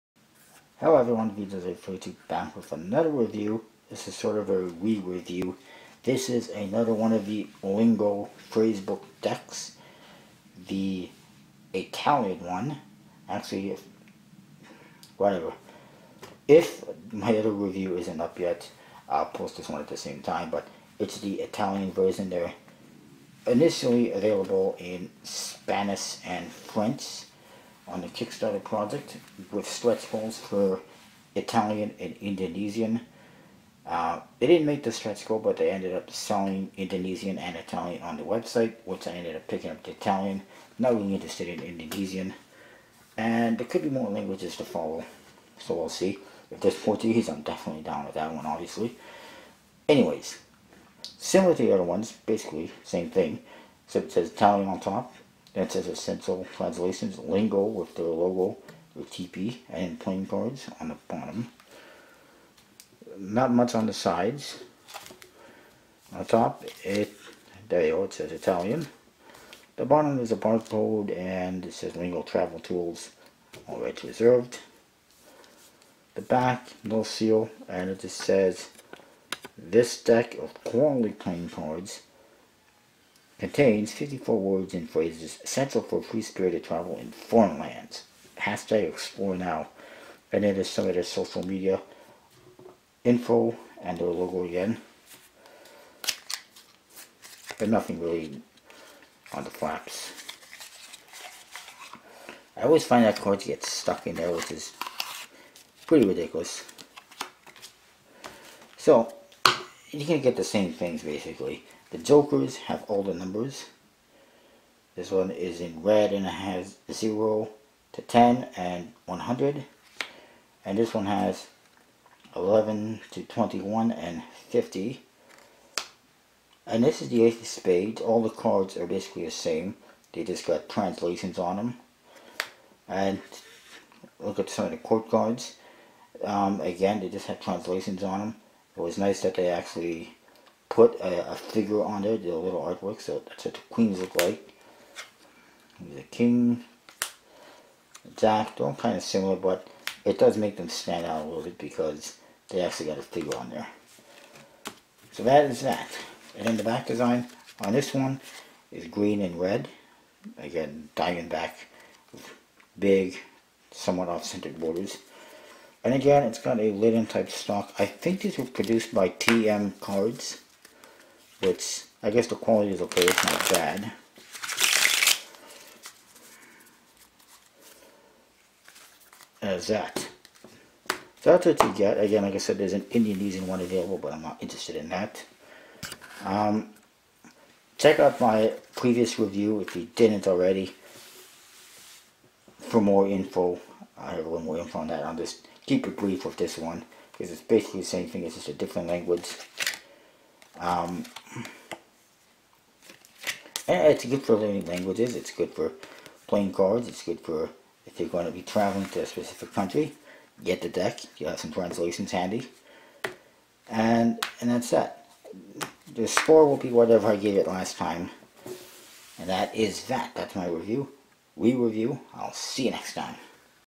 Lingo Italian Deck Review
If you say the previous review this is the same except in a different language and color but I thought I would show it to you again quickly.

The code vjose32 also works great at richiescardstore.com and cartesmagie.com for more discounts!